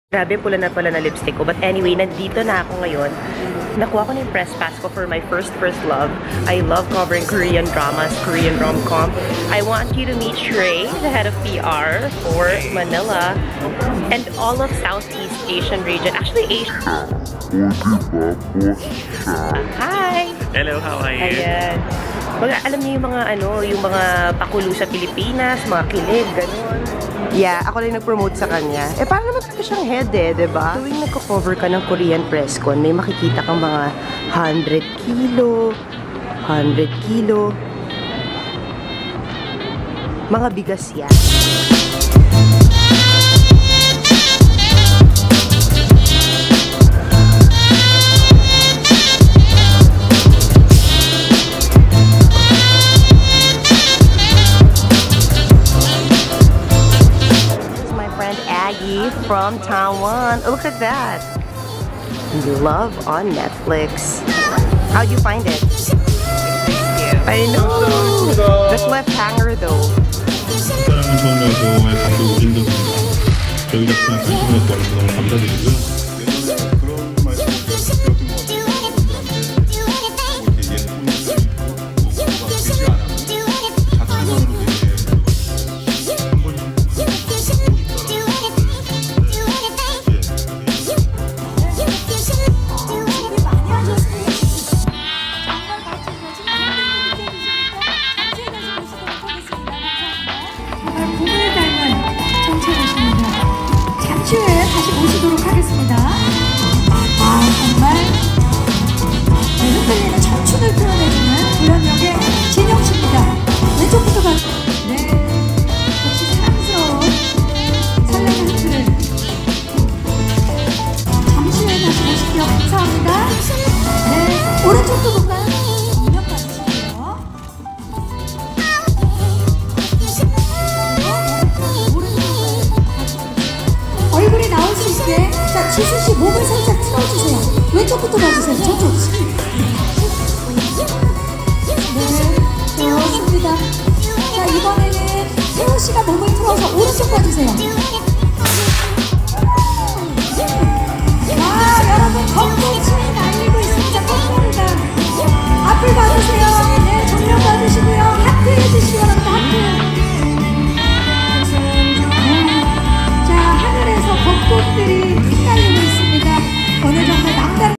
Behind the scenes w/ Jisoo, Jinyoung, Jung Chae-yon & cast in new Netflix series original!
We covered the stars of Netflix’s first ever romantic comedy Korean original, My First First Love in Seoul. It’s showing on April 18. Cast includes Ji Soo, Jinyoung, Jung Chae-yon.